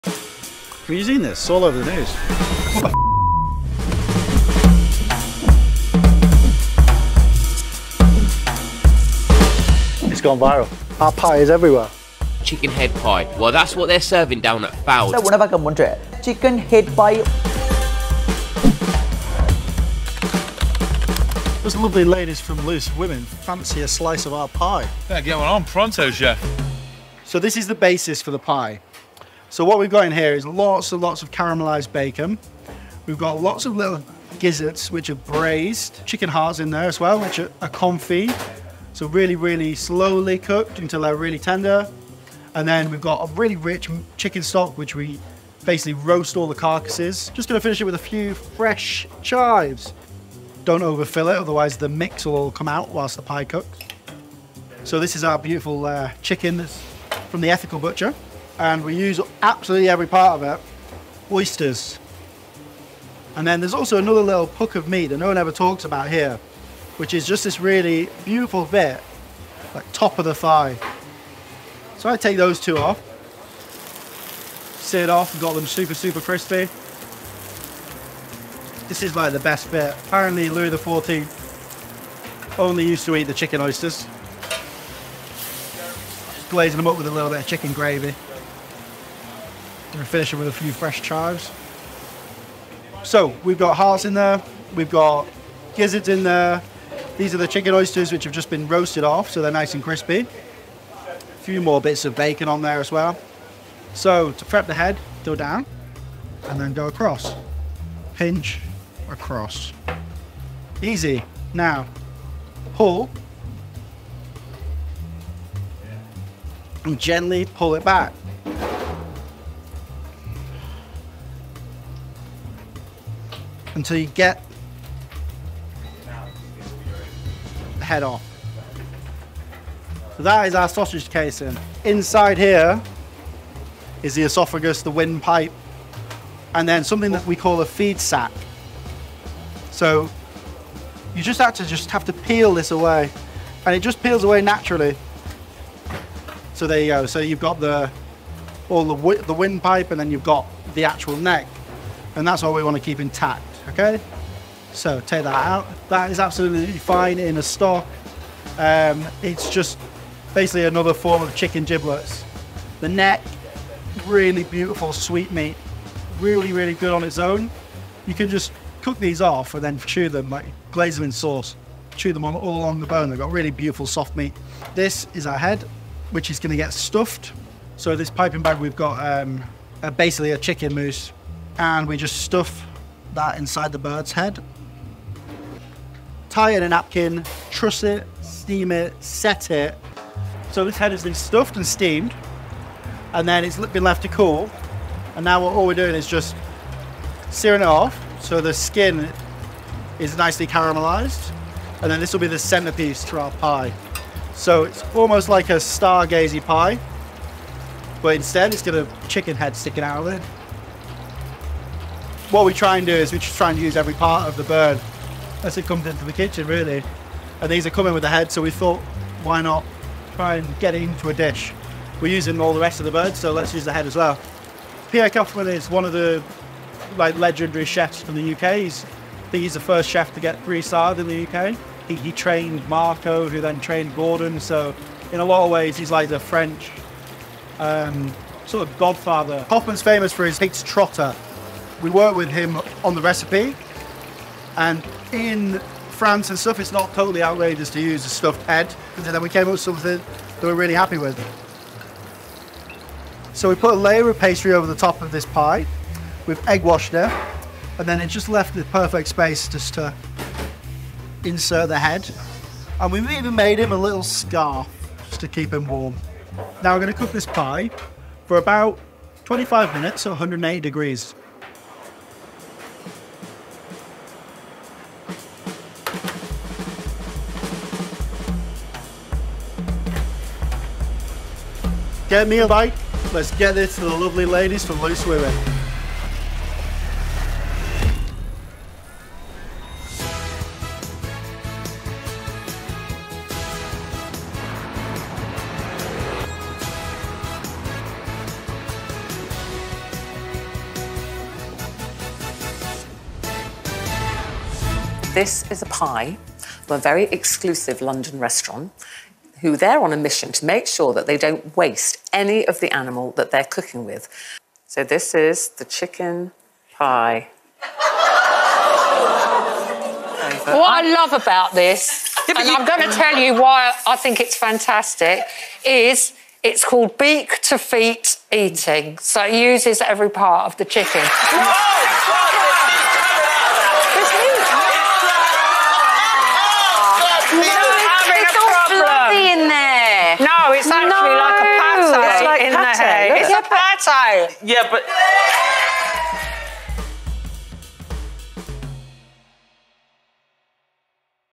Our Pie Went Viral...
Our pie has officially broke the internet...

We're about to unravel the extraordinary story of the viral chicken head pie that has captured the attention of food lovers worldwide. Made in collaboration with the legend Pierre Koffmann, our pie has been making headlines on news outlets and social media around the globe, is now making its way to the set of the popular TV show "Loose Women." We're rushing a special order to showcase this unique culinary sensation.

In this video, we'll delve into the creative process, recipe and secret ingredients behind this viral sensation. Join us as we take you behind the scenes to uncover the magic that made this pie an internet sensation.

Whether you're a food enthusiast, a fan of viral sensations, or just curious about the latest food trends, this video is a must-watch.

'Fallow restaurant is a Contemporary British restaurant serving innovative food and carefully sourced ingredients'